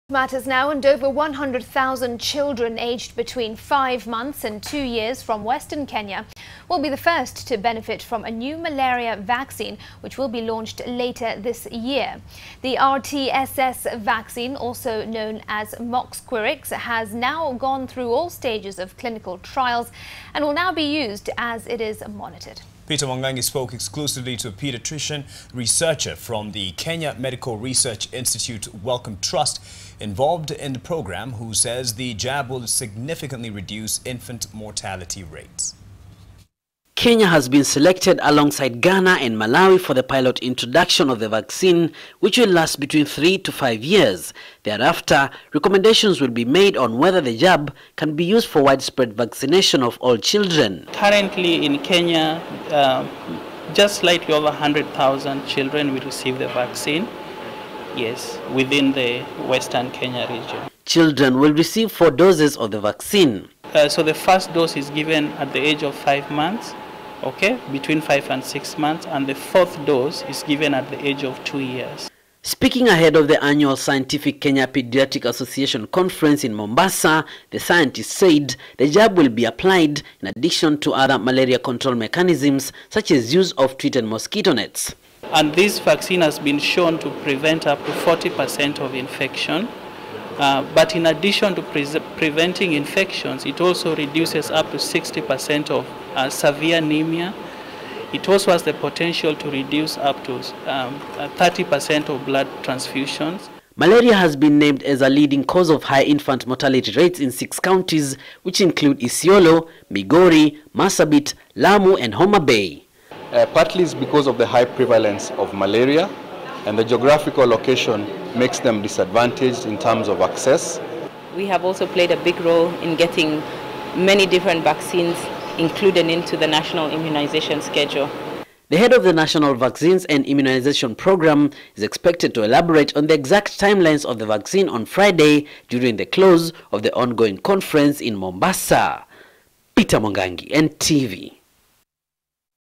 KEMRI scientists say malaria vaccine will be launched this year
Over 100,000 children, aged between five months and two years, from Western Kenya, will be the first to benefit from a new malaria vaccine, which will be launched later this year.

The RTSS vaccine, also known as Mosquirix, has now gone through all stages of clinical trials, and will now be used as it is monitored. Peter Mwangangi spoke exclusively to a paediatrician researcher from the Kenya Medical Research Institute (KEMRI) Wellcome Trust involved in the program, who says the jab will significantly reduce infant mortality rates.